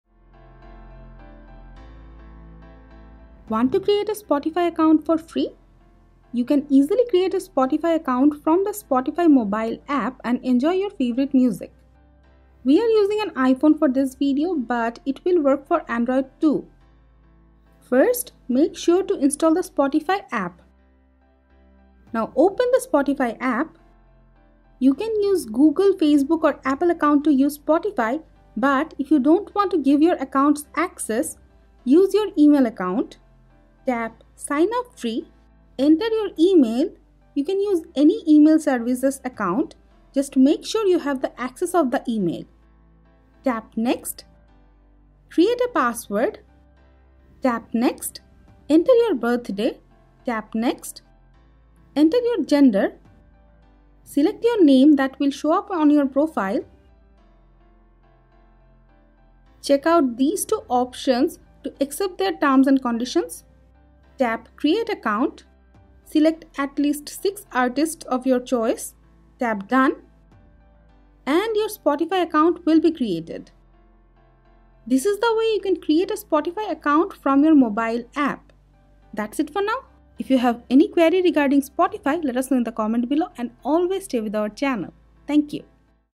Create a FREE Spotify Account on iPhone & Android (2022)
Looking for a way to Create a Spotify Account? In this vidoe wewill show you the Step by step process to CreateSpotifyAccount from iPhone or Android.